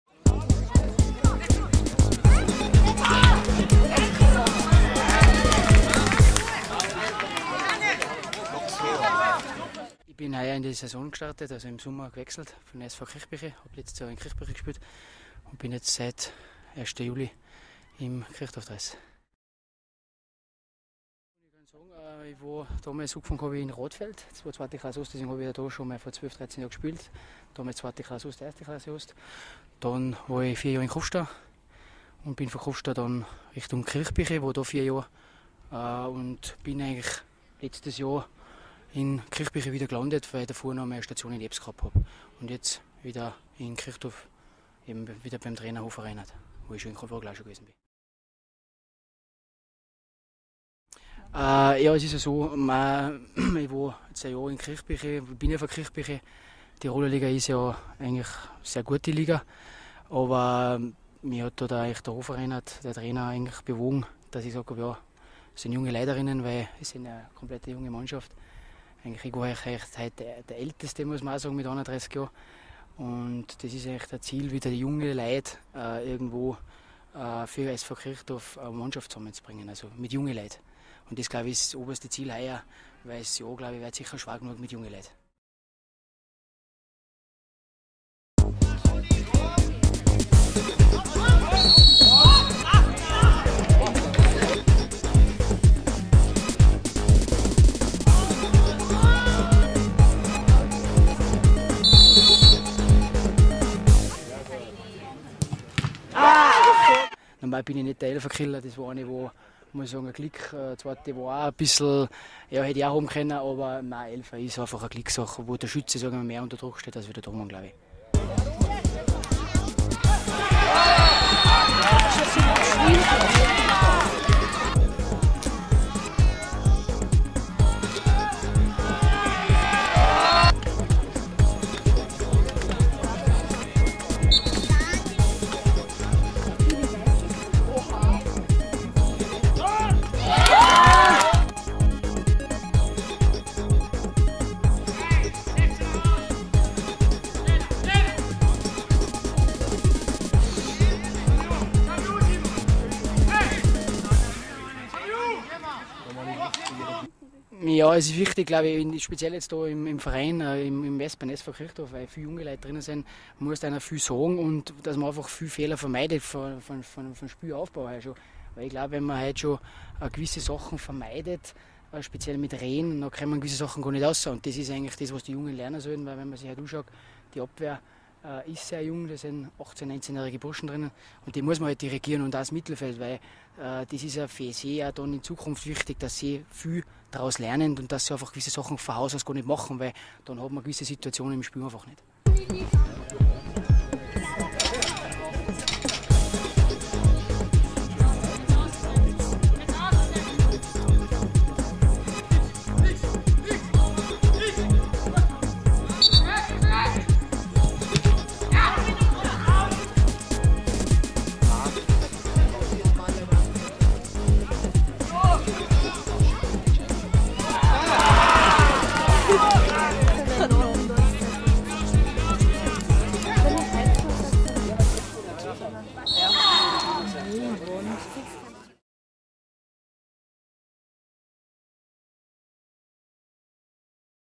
Tormann Christian Fercher SV Kirchdorf
Christian Fercher ist mit Juli 2011 der neue Tormann des Landesligisten SV Kirchdorf. Trainer Reinhard Hofer hat ihn von Kirchbichl geholt, nicht zuletzt um seiner sehr jungen Mannschaft die nötige Routine und Sicherheit für die Hintermannschaft zu geben.